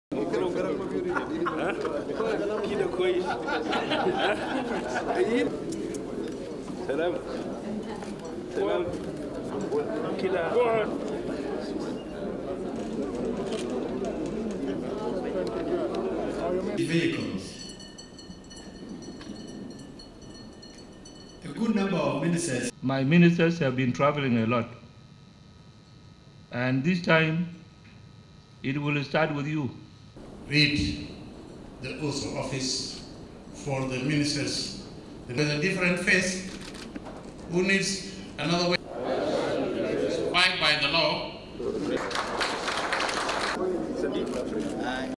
Cabinet ministers sworn in, Corona virus precautions announced
In mid-March before COVID-19 had reached South Sudan, the global existence of and threat posed by the virus left its mark at the swearing in of the cabinet of the transitional government of national unity: 35 ministers (eight women) and 10 deputies took their oaths of office.